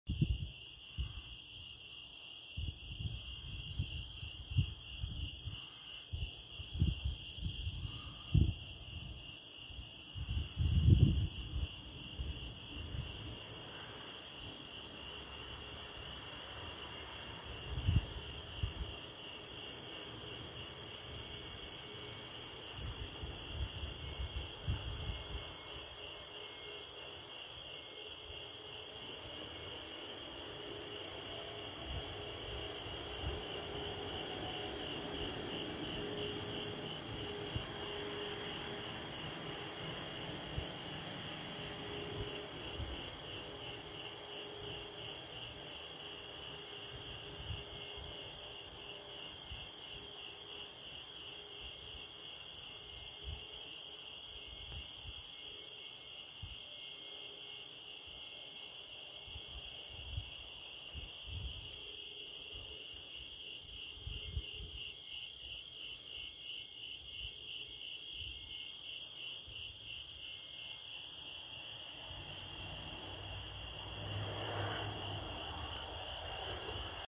Almost Halloween
Spider has been working on the web for over an hour.  Don't know what kind of  Spider it is..